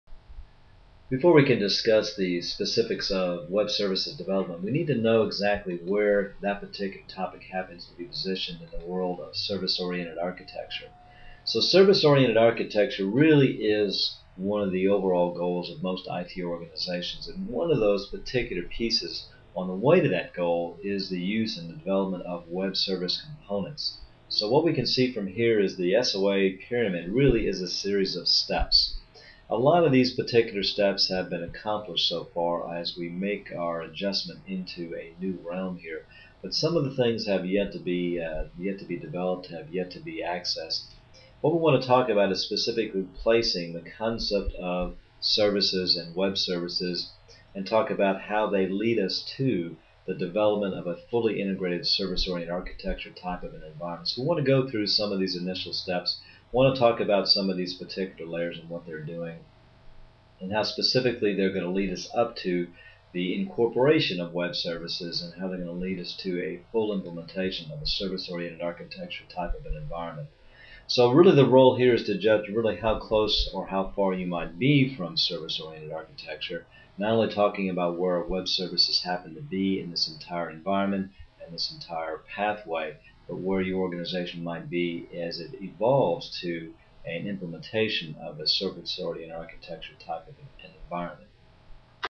Service-Oriented Architecture Pyramid
Illustrates the underlying principles and technologies that comprise a Service-Oriented Architecture framework